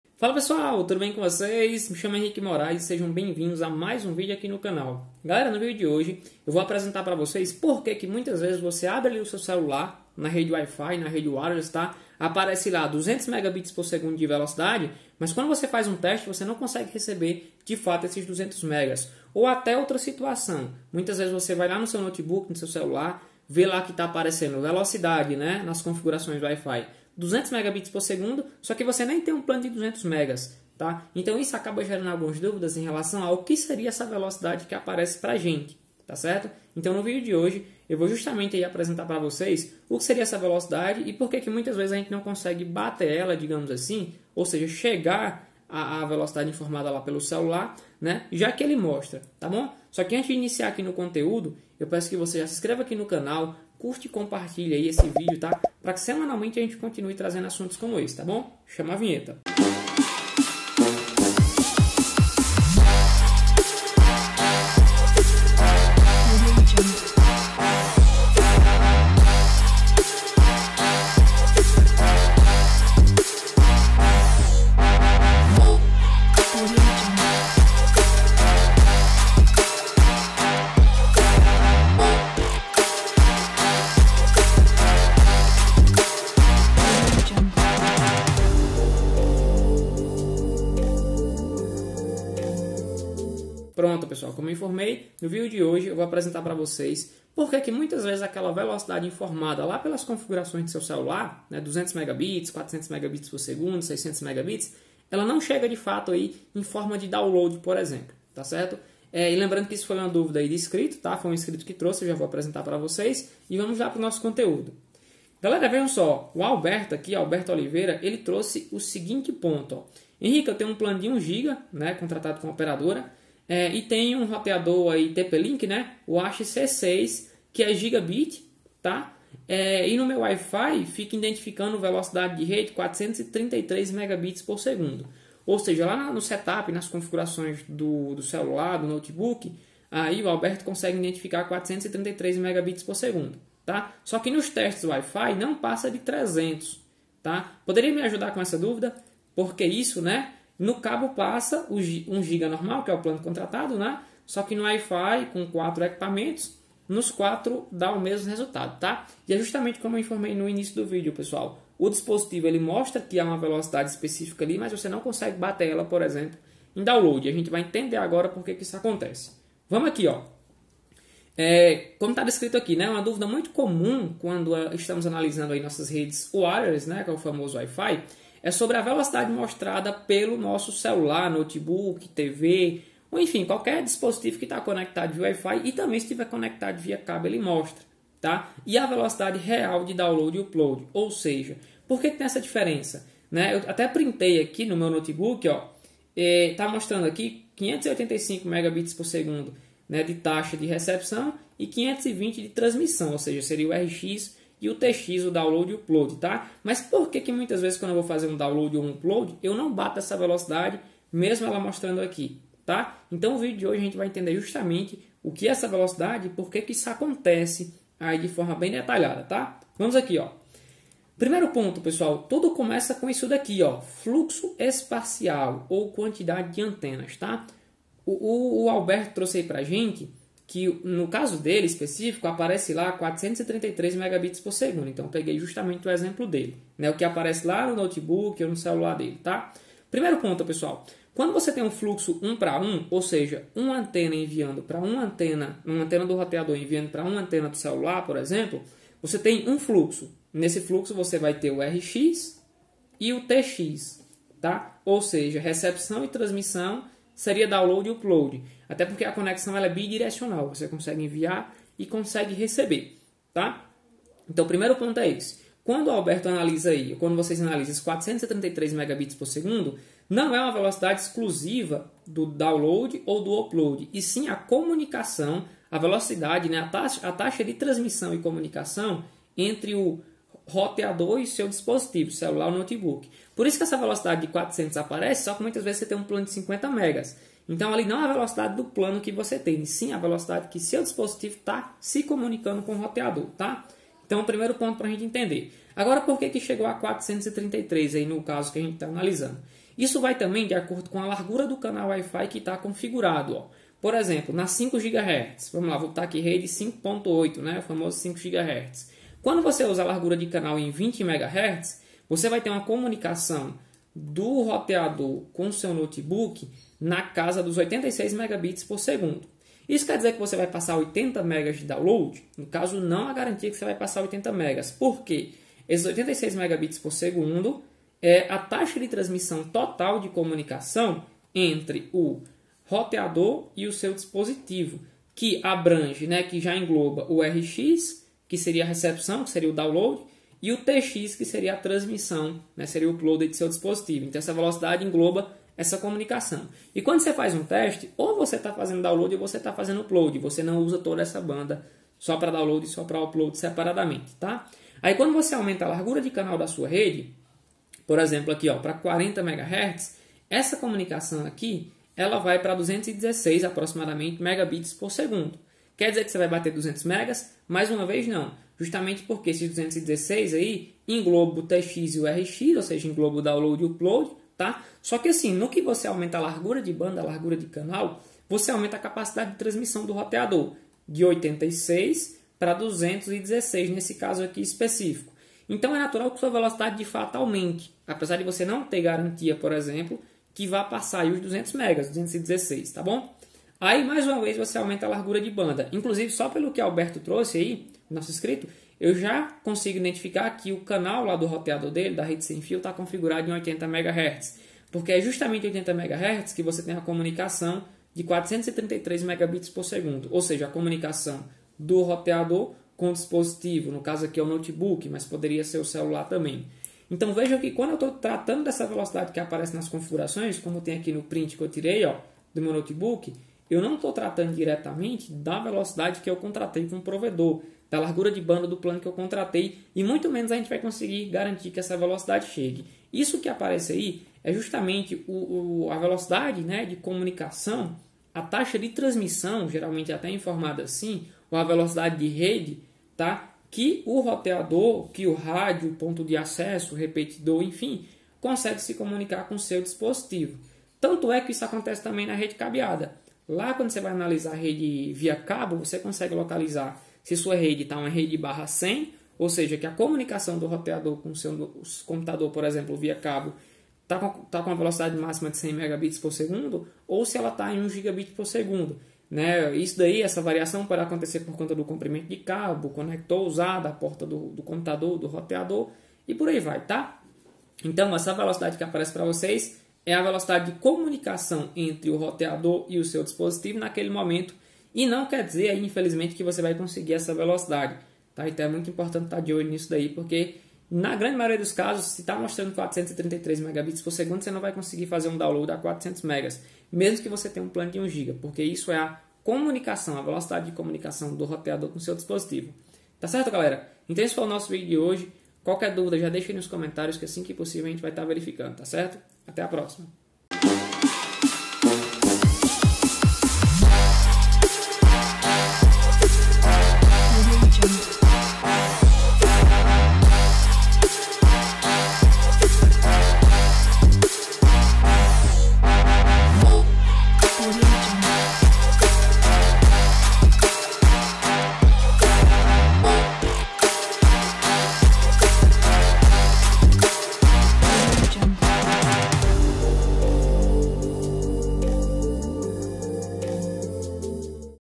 433Mbps de link no celular mas não chega o plano contratado - Entenda o motivo
No vídeo de hoje iremos analisar o motivo pelo qual o seu celular no suporta o plano contratado, mesmo tendo suporte a velocidades maiores.